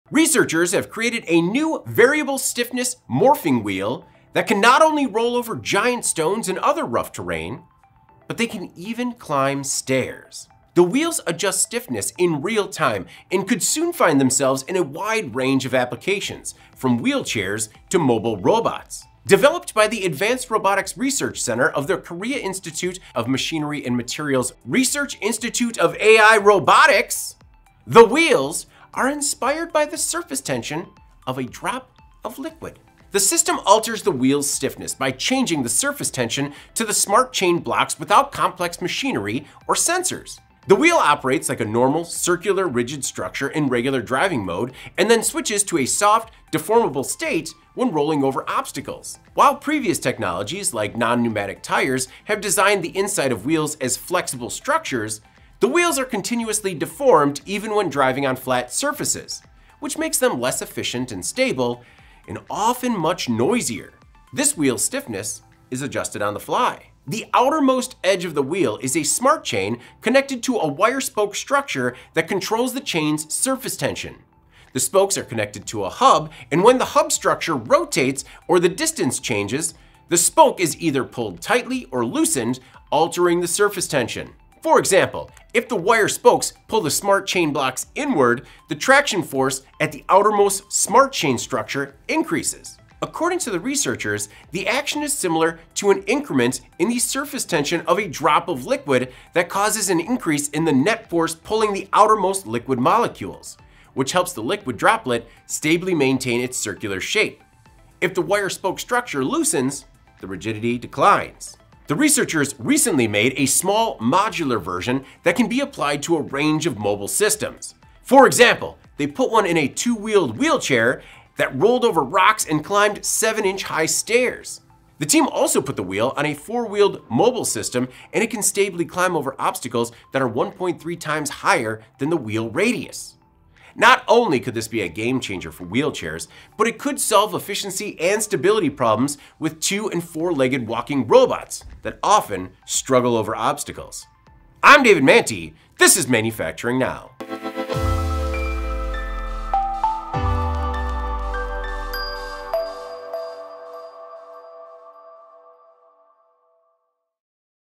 Morphing Wheels Can Easily Drive Up Stairs
Researchers have created a new variable-stiffness morphing wheel that can not only roll over giant stones and other rough terrain, but they can even climb stairs. The wheels adjust stiffness in real time and could soon find themselves in a wide range of applications, from wheelchairs to mobile robots.